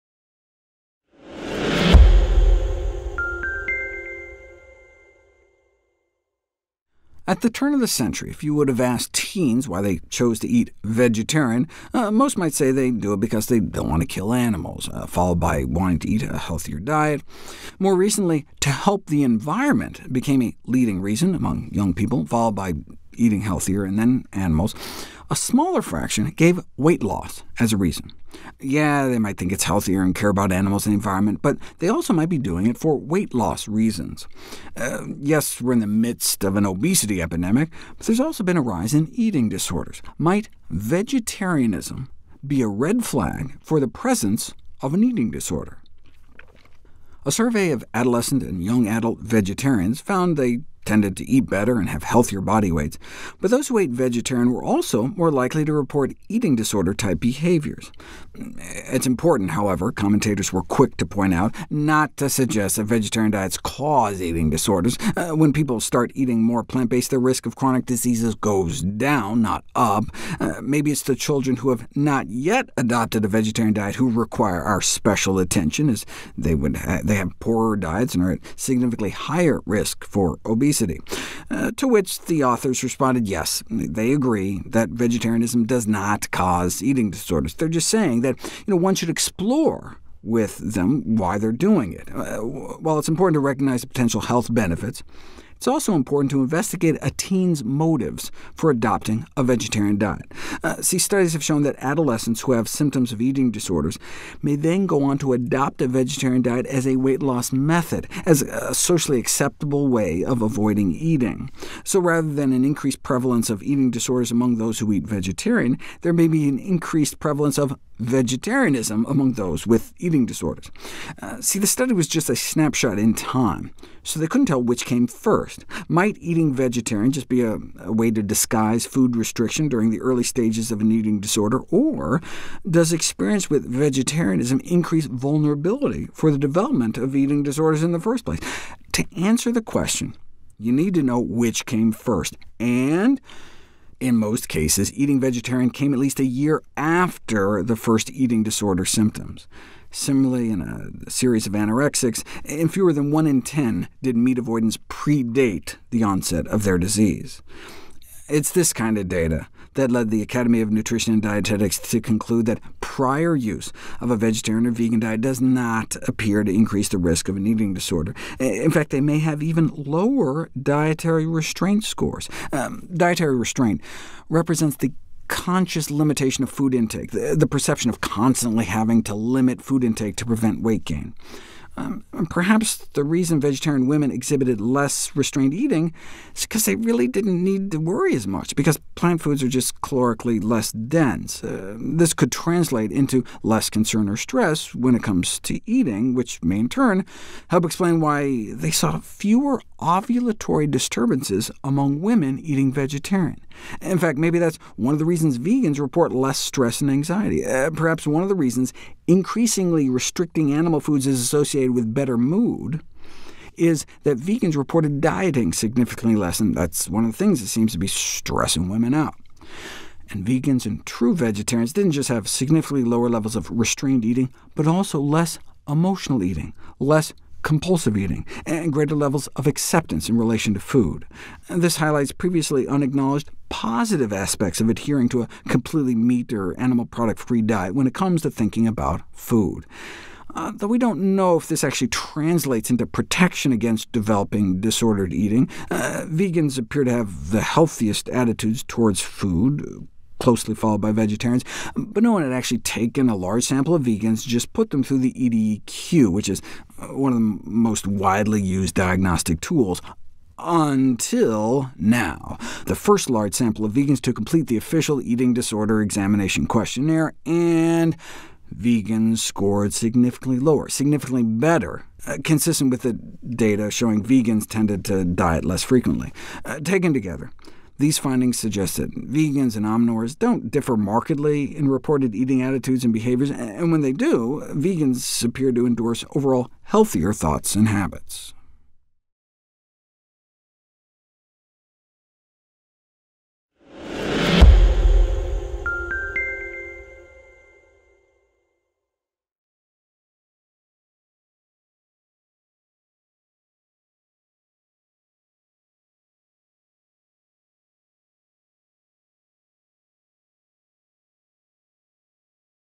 Jsou vegetariáni ohroženi poruchami příjmu potravy?
Kdybyste se na přelomu století zeptali dospívajících, proč se rozhodli stravovat vegetariánsky, většina by vám řekla, že to udělali proto, že nechtěli zabíjet zvířata. Hned po tomto důvodu následovala snaha stravovat se zdravěji. V nedávné době se stala důležitým důvodem snaha „pomoci životnímu prostředí,“ která je oblíbeným důvodem k vegetariánství u mladých lidí. Následuje zdraví a pak zvířata. Menší zlomek dotazovaných uvedl jako svůj důvod hubnutí. Mohou si myslet, že je to zdravější životní styl a může jím záležet na zvířatech i životním prostředí, ale mohou to dělat také za účelem zhubnout. Ano, jsme uprostřed epidemie obezity, ale také došlo k nárůstu poruch příjmu potravy. Může být vegetariánství varovným znakem ohrožení jedince poruchou příjmu potravy?
Průzkum na dospívajících a mladých dospělých vegetariánech zjistil, že obecně jedli zdravěji než běžní lidé a měli zdravější tělesnou hmotnost. Zároveň však vegetariáni častěji hlásili chování typické pro poruchy příjmu potravy. „Je však důležité,“ poznamenali rychle komentátoři, „nepovažovat toto za znak toho, že by vegetariánská strava způsobovala poruchy příjmu potravy.“ Když lidé začnou upřednostňovat rostlinnou stravu, jejich riziko chronických onemocnění nestoupne, nýbrž klesne. Možná jsou to spíše děti, které si ještě neosvojily vegetariánské stravování, kdo vyžaduje naši zvláštní pozornost, „protože mají na živiny chudší stravu a mají výrazně vyšší riziko obezity.“
Na to autoři odpověděli, ano, „souhlasíme s tím, že vegetariánství nezpůsobuje poruchy příjmu potravy.“ Oni jen říkají, že byste měli prozkoumat důvody, proč to daný člověk dělá. „I když je důležité uznat možné zdravotní přínosy, je také důležité zkoumat motivy mladistvých k přijetí vegetariánské stravy.“ Vidíte, „studie ukázaly, že dospívající, kteří projevují příznaky poruch příjmu potravy, se mohou uchýlit k vegetariánství jako k prostředku k dosažení požadované váhy, jelikož je to společensky přijatelný způsob, jak se vyhnout konzumaci jídla.“ Takže spíše než zvýšený výskyt poruch příjmu potravy mezi vegetariánsky se stravujícími lidmi může jít o zvýšený výskyt vegetariánství mezi těmi, kteří mají poruchy příjmu potravy.
Víte, ta studie sbírala jen informace platné v daném okamžiku, nemohli tedy podle nich poznat, co z toho přišlo jako první. Může být vegetariánské stravování prostě jen „způsob, jak zamaskovat omezení příjmu potravy v časných fázích poruchy příjmu potravy? Nebo snad zkušenosti s vegetariánstvím zvyšují sklony k rozvoji poruch příjmu potravy?“ Chcete-li na tuto otázku znát odpověď, musíte vědět, k čemu došlo dříve. Ve většině případů vegetariánská strava přišla po minimálně jednom roce od objevení prvních příznaků poruchy příjmu potravy. Podobně i u skupiny pacientů s anorexií pouze u necelé jedné desetiny z nich vyřazení masa předcházelo nástupu onemocnění.
Poznatky, jako jsou tyto, dovedly Academy of Nutrition and Dietetics k závěru, že „předcházející dodržování vegetariánské nebo veganské stravy se nezdá být jevem zvyšujícím riziko vzniku poruchy příjmu potravy.“ Ve skutečnosti tito lidé mohou svůj příjem potravy omezovat méně. „Omezení příjmu potravy představuje vědomé omezení množství konzumovaného jídla,“ pocit, že si musí člověk neustále hlídat svůj kalorický příjem, aby tak zabránil přírůstku hmotnosti. Možná je důvodem, proč vegetariánské ženy vykazovaly méně omezování jídla, že si s tím skutečně nemusely dělat tolik starostí, protože rostlinné potraviny zkrátka obsahují na svou váhu méně kalorií. „To se mohlo projevit menším výskytem obav nebo stresu z konzumace jídla,“ což může vysvětlit, proč u vegetariánských žen následně pozorovali méně poruch ovulace. Možná je to dokonce důvodem, proč vegani uvádějí méně pocitů stresu a úzkosti. Možná je jedním z důvodů, proč je větší omezování živočišných potravin spojeno s lepší náladou, to, že vegani dle výpovědí méně dodržují diety. A diety jsou jedna z věcí, které ženy stresují.
Vegani a skuteční vegetariáni nejen mnohem méně omezovali svůj příjem potravy, ale také méně jedli z důvodu emocionálního uspokojení, méně trpěli nutkavým přejídáním a měli lepší vztah k jídlu. „To jen zdůrazňuje dříve nepotvrzené pozitivní stránky dodržování stravy vyřazující maso nebo všechny živočišné potraviny“ z hlediska zdravého nahlížení na jídlo. Přesto nevíme, jestli se to skutečně projeví jako ochrana proti rozvoji poruch příjmu potravy. „Zdá se, že vegani mají nejzdravější postoje k jídlu, těsně za nimi jsou vegetariáni.“